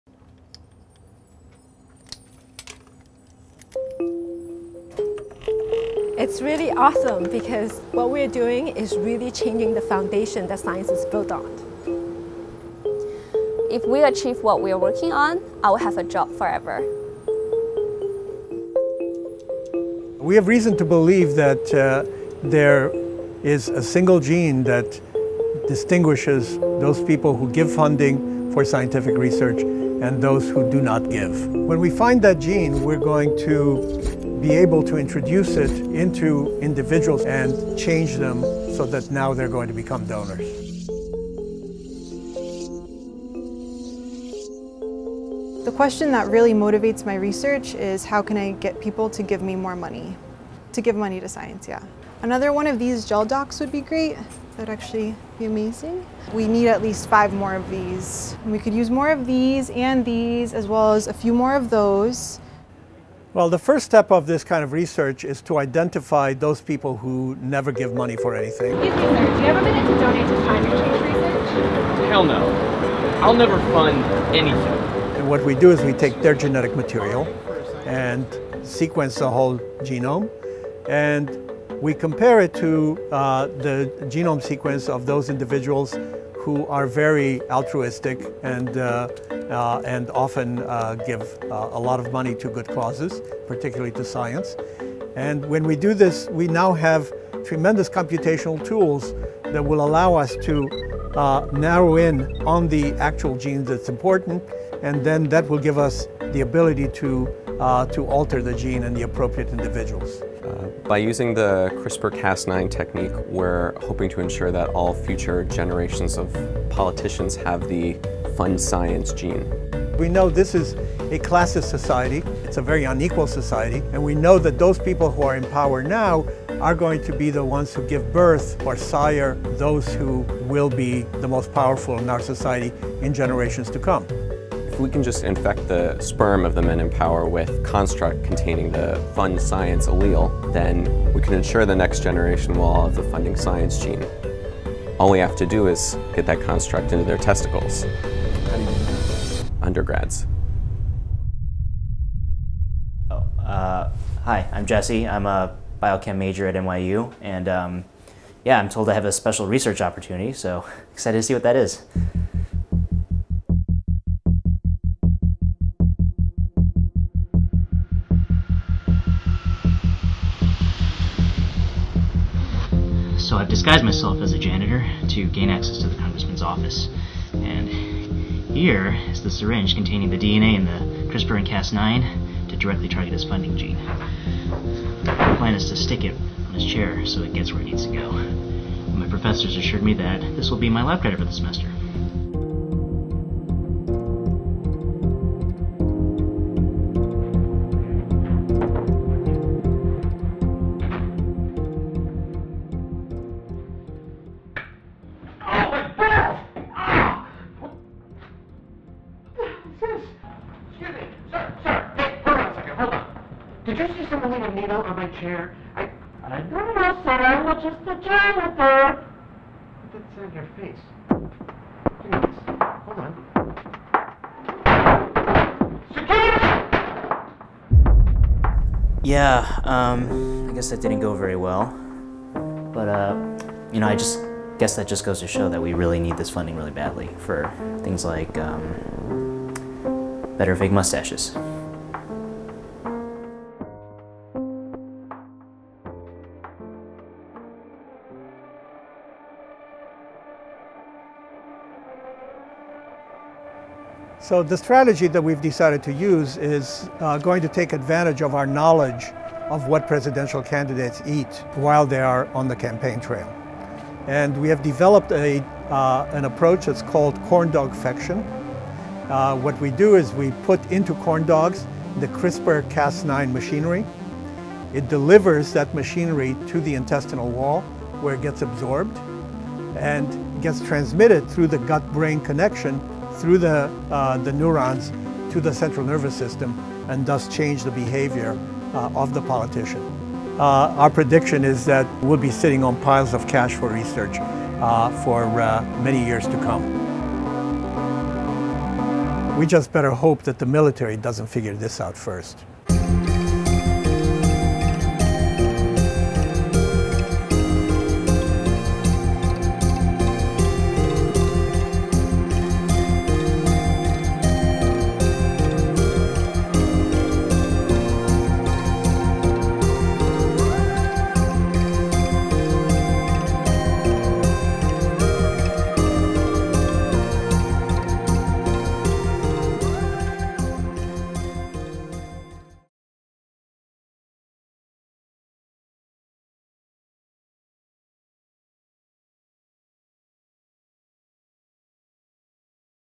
Funding Fever
The inside story of the scientists attempting to manipulate the gene that determines if a human will give funding to science.

Created for the American Association of Immunologists 2015 Annual Meeting

Written And Directed by Matt Klinman
Shot and Edited by Alec Cohen